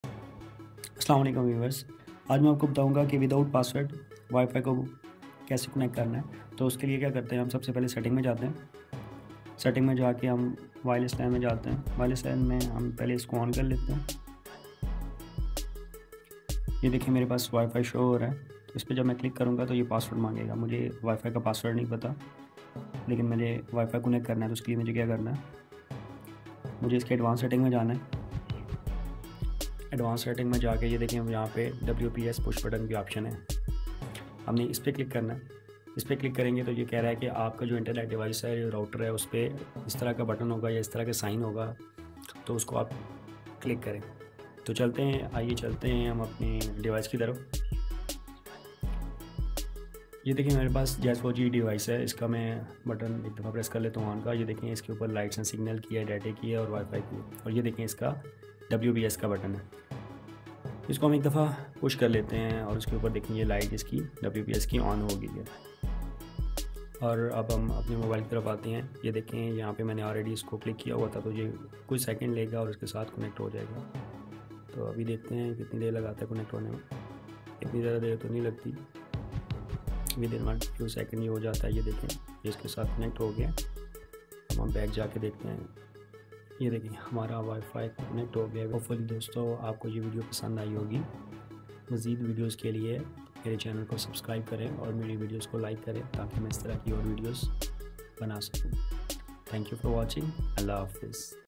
How to connect Wifi without password
In this video, I'm going to show you how to connect wifi without a password.
The Wi-Fi Push Setup (WPS) Key enables you to connect to the device without entering the password.

Simply press the WPS key for 3 seconds or one time depends on your router functionality and then press the WPS button/option on your mobile phone, tablet or computer.

Every day, I will upload a video that is informative for you.